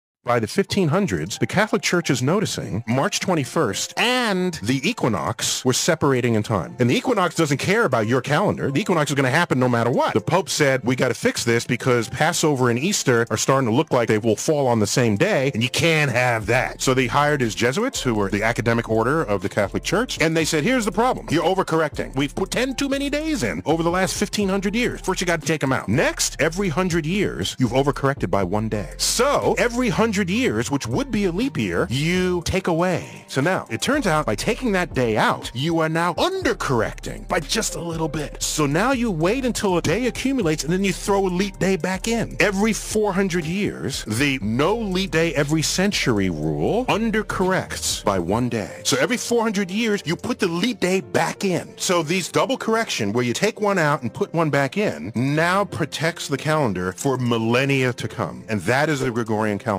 Why Does Our Calendar Skip Leap Years Every 100 Years? 🤯 Neil deGrasse Tyson
Ever wondered why leap years aren’t every 4 years like you were taught? Neil deGrasse Tyson dives into the wild history of the Gregorian calendar — from ancient mistakes to Jesuit astronomers fixing time itself. Discover why we skip leap years every century but don’t skip them every 400 years! It’s all about aligning with the Sun and the seasons.